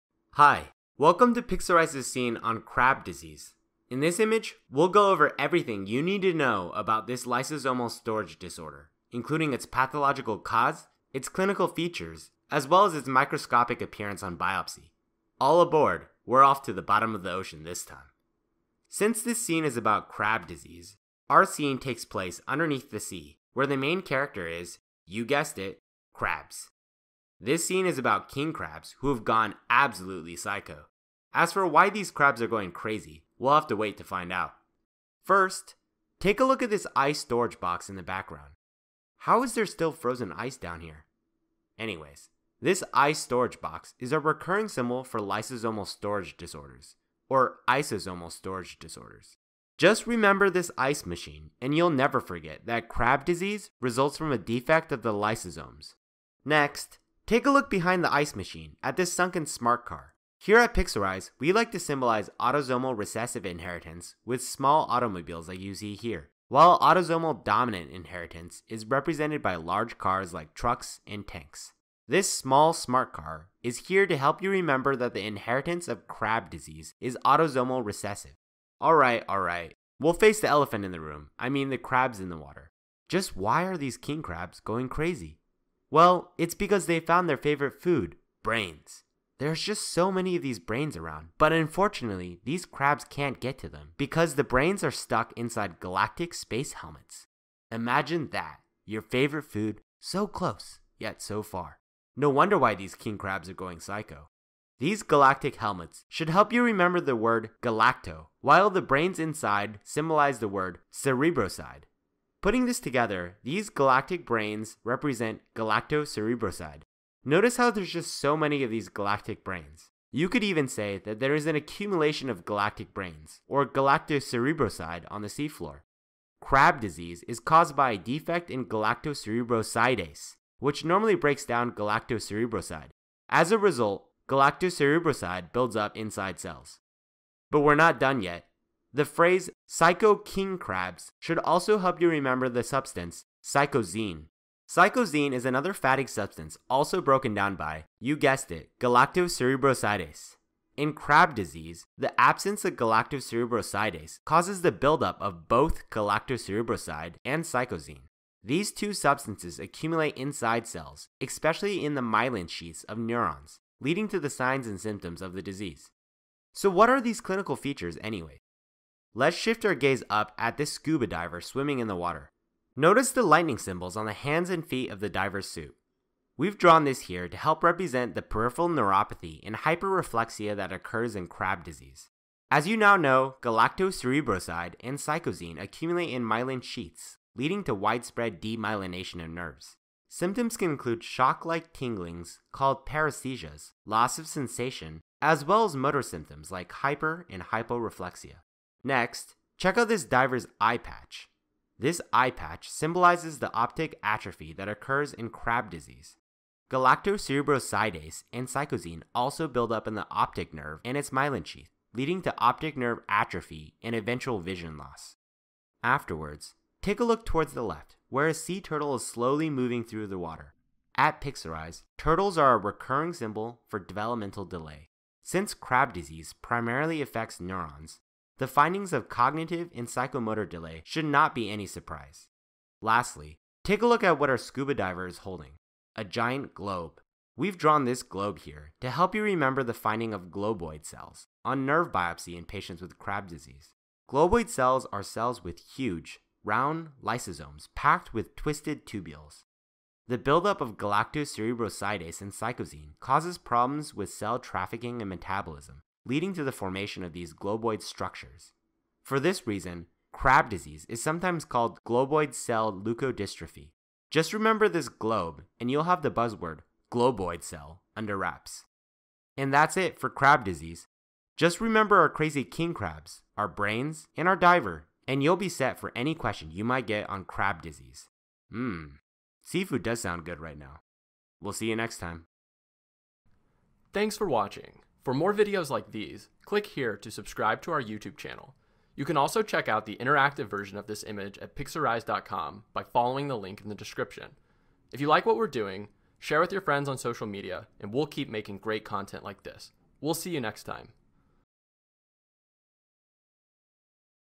Krabbe Disease | Lysosomal Storage Disease Mnemonic
Krabbe disease, also known as globoid cell leukodystrophy, is an autosomal recessive lysosomal storage disease characterized by a number of neurological signs and symptoms. It is caused by a mutation in galactocerebrosidase (GALC), which leads to an accumulation of the sphingolipids galactocerebroside and psychosine. Krabbe disease is therefore one of the sphingolipidoses, as the accumulation of psychosine leads to oligodendrocyte destruction and subsequent demyelination. It is also considered a leukodystrophy (hence "globoid cell leukodystrophy"), which is characterized by white matter degeneration in the brain. Leukodystrophies are caused by abnormal myelin development, which in Krabbe disease is secondary to psychosine accumulation. Unsurprisingly, Krabbe disease patients present with a number of neurological findings. This includes peripheral neuropathy, the signs and symptoms of which include hyper- and hypo-reflexia in addition to paresethesias. Similarly, optic nerve involvement can lead to optic atrophy and blindness.

Go under the sea to learn about Krabbe Disease with Pixorize's visual mnemonics for med school. Part of our Lysosomal Storage Diseases playlist for biochemistry and the USMLE Step 1 exam.